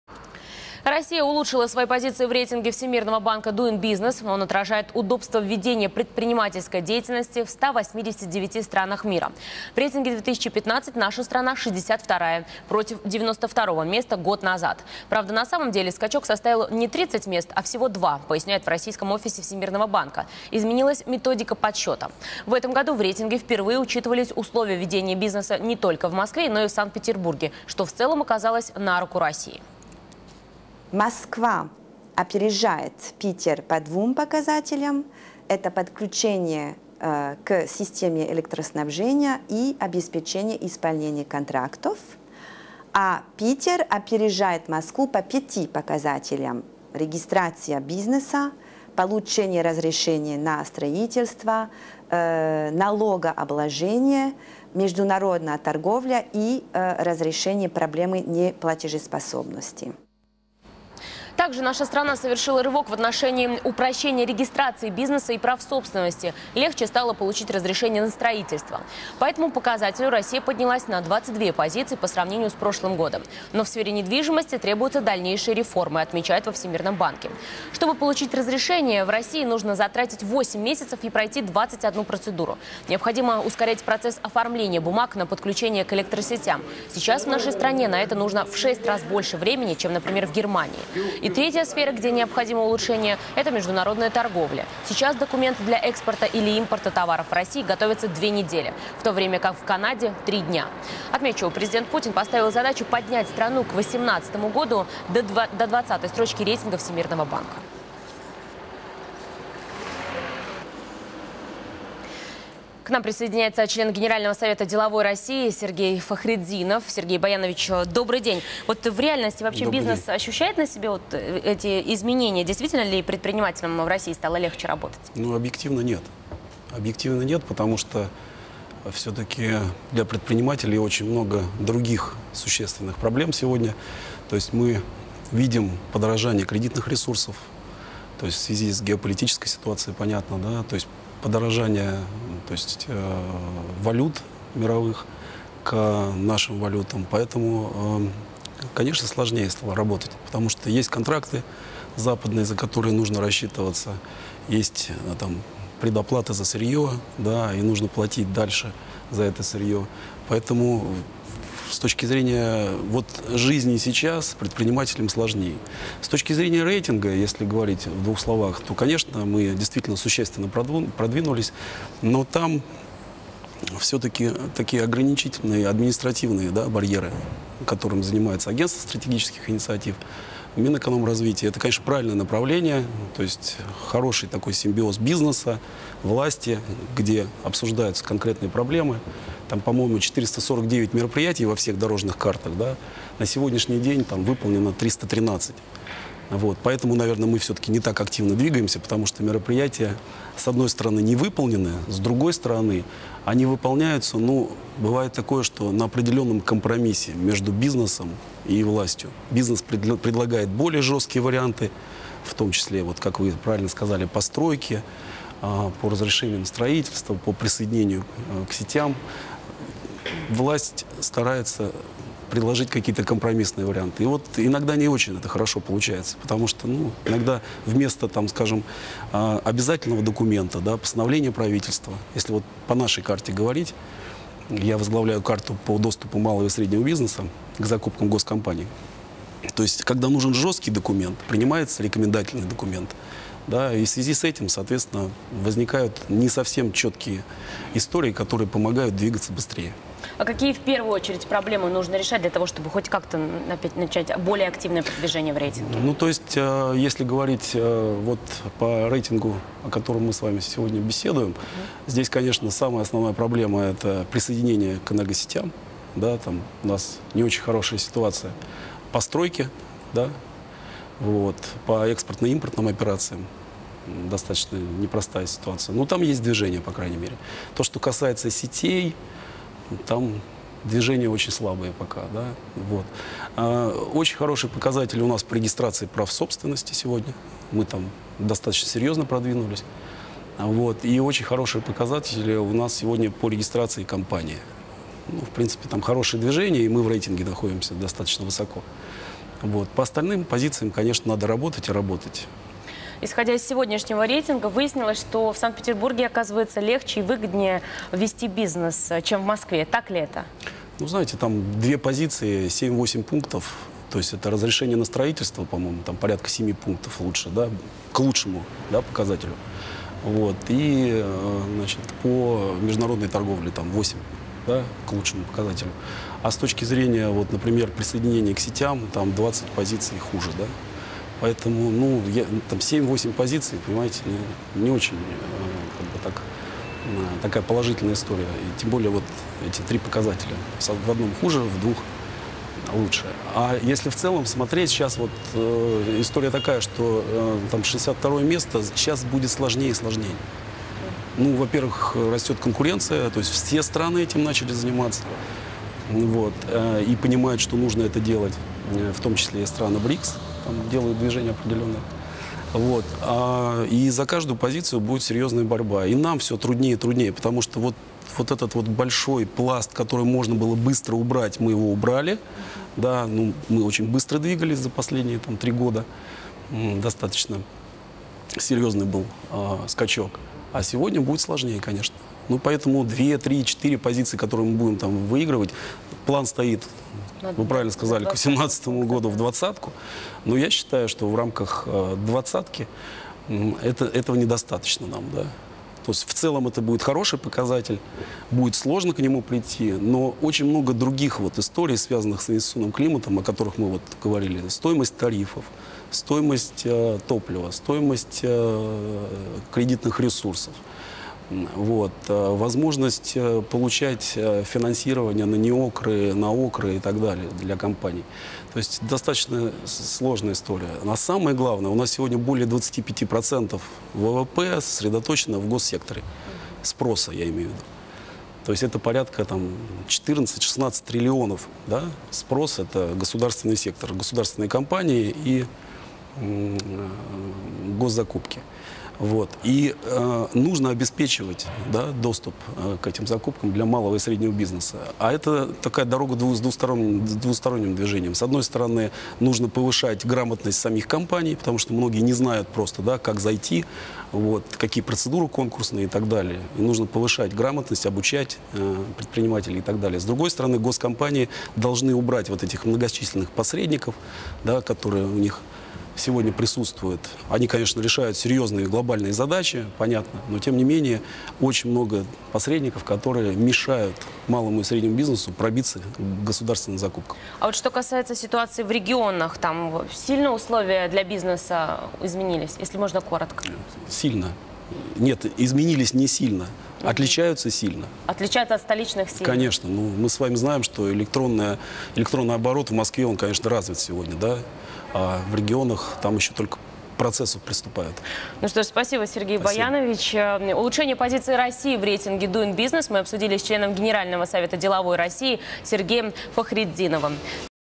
Сергей Фахретдинов о результатах Doing Business 2015
Россия улучшила свои позиции в рейтинге Всемирного банка Doing Business.

Он отражает удобство ведения предпринимательской деятельности в 189 странах мира. В рейтинге-2015 наша страна 62-ая, против 92-ого места год назад. Правда, на самом деле скачок составил не тридцать мест, а всего 2, поясняют в российском офисе Всемирного банка, изменилась методика подсчёта. В этом году в рейтинге впервые учитывались условия ведения бизнеса не только в Москве, но и в Санкт-Петербурге, что в целом оказалось на руку России.